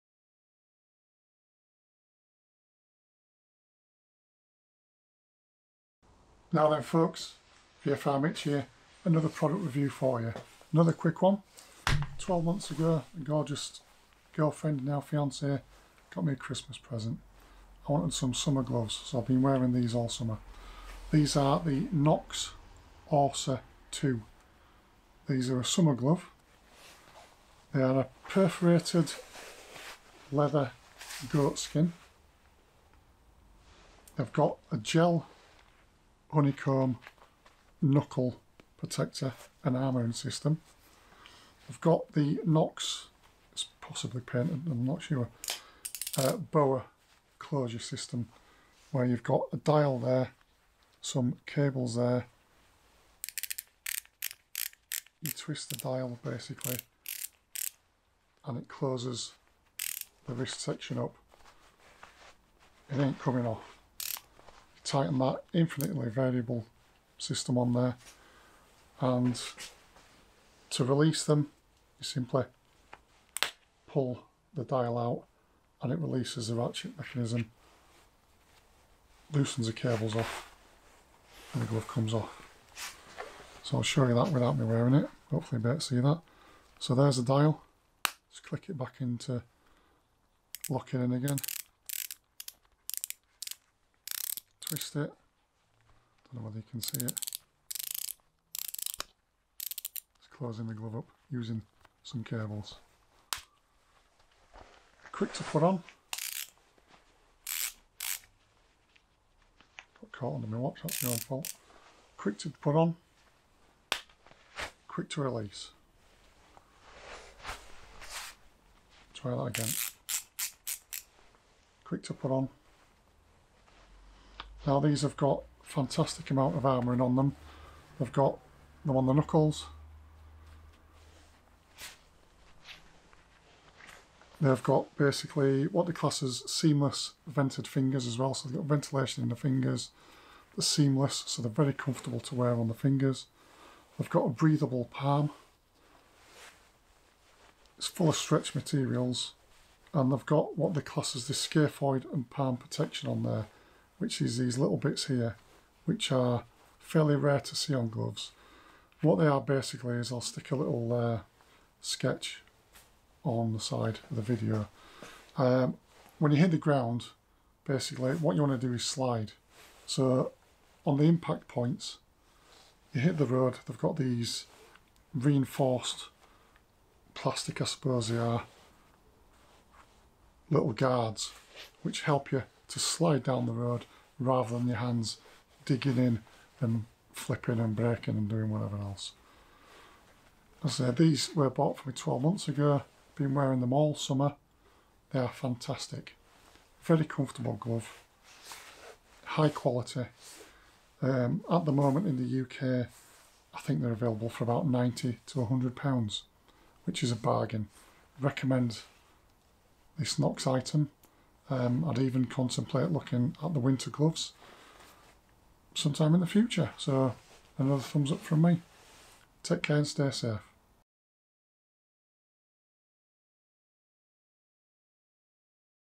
Knox Motorbike gloves - Orsa 2 - Review
A short review of my summer riding gloves my Knox Orsa II.
A superbly comfortable high quality safe pair of short style summer gloves. I love them.